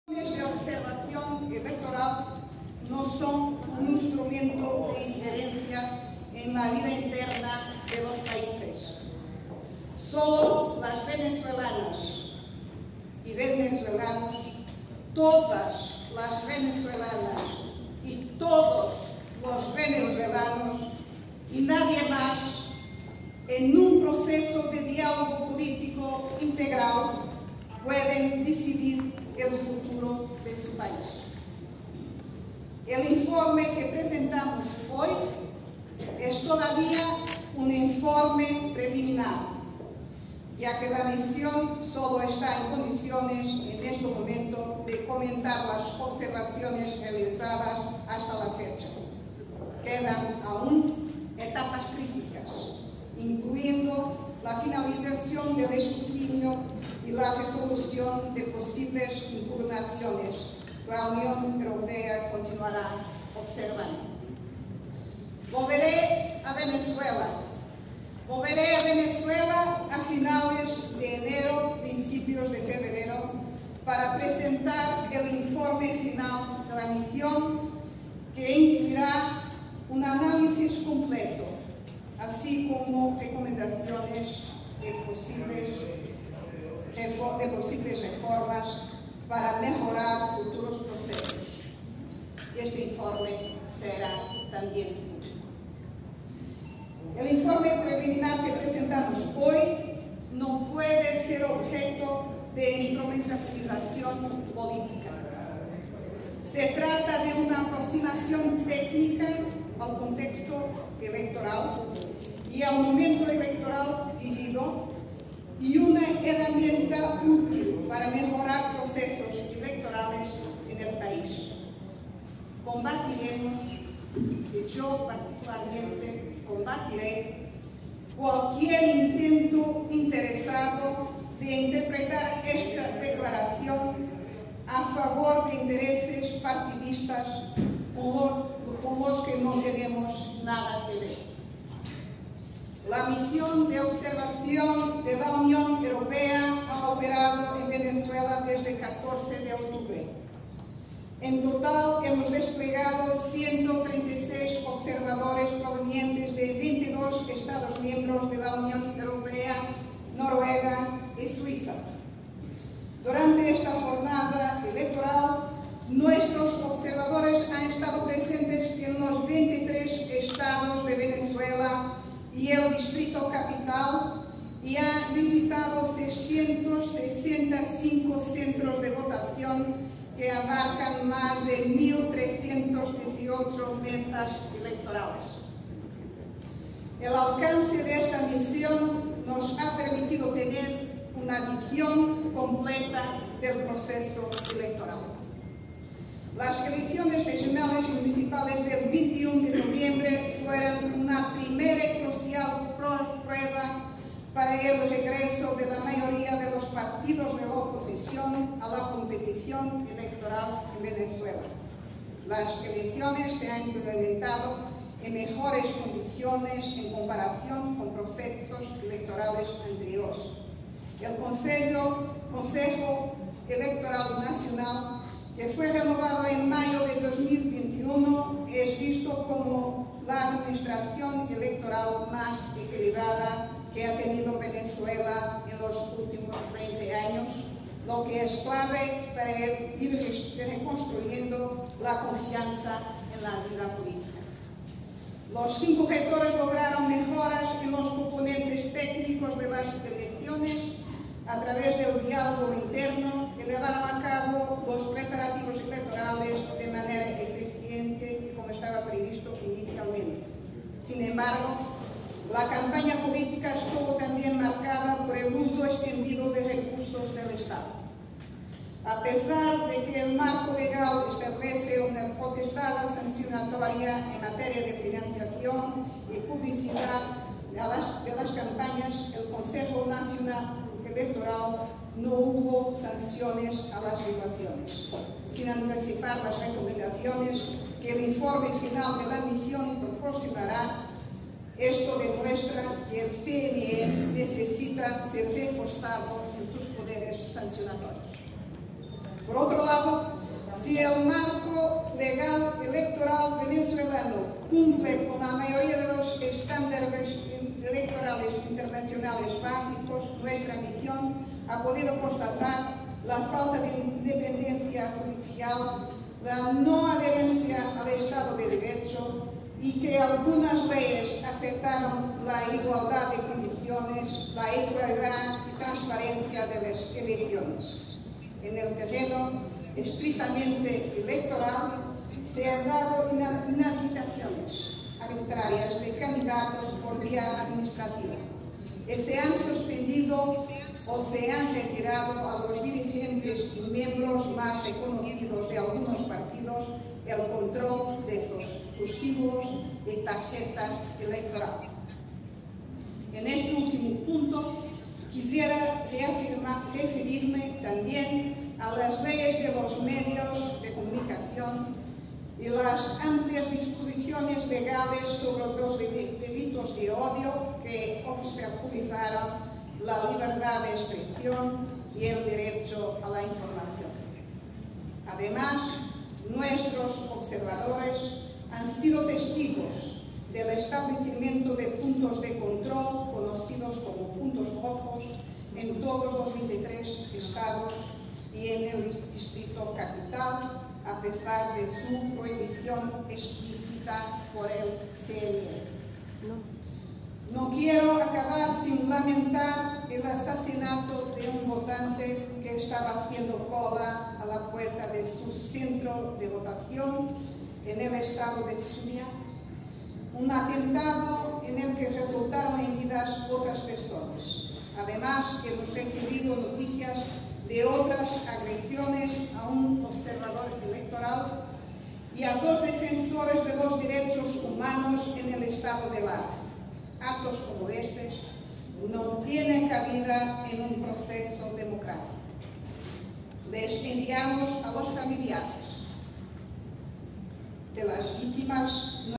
Isabel Santos (UE): La campaña estuvo marcada por el uso extensivo de recursos del Estado
video cortesía Juan Carlos Neira